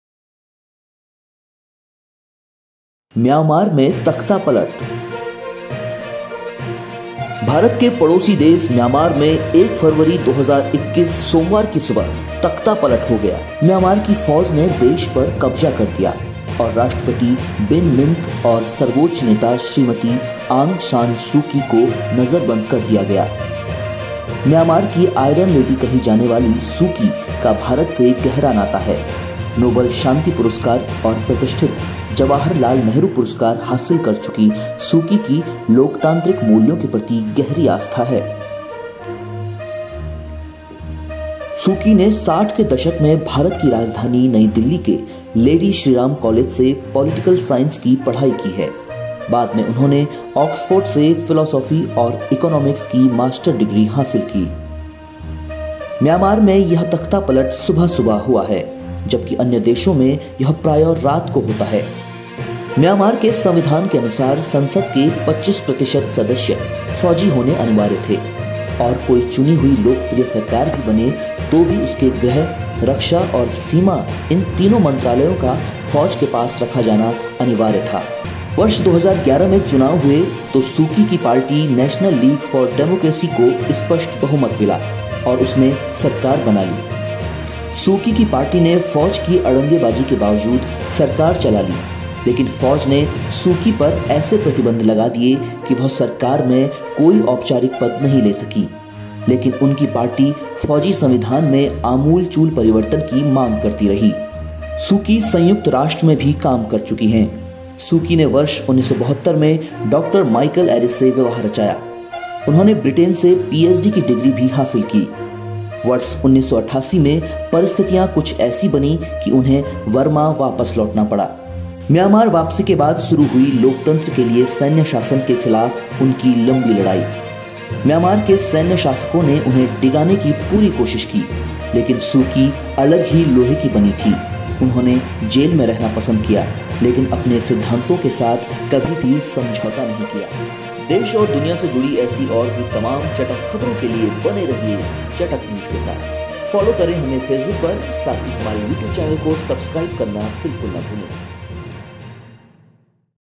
Myanmar में सेना ने किया तख्तापलट, नेता Aung San Suu Kyi और राष्ट्रपति हिरासत में
Myanmar में सेना ने किया तख्तापलट, नेता Aung San Suu Kyi और राष्ट्रपति हिरासत में चटक_News MyanmarArmy​ AungSanSuuKyi​ MyanmarEmergency​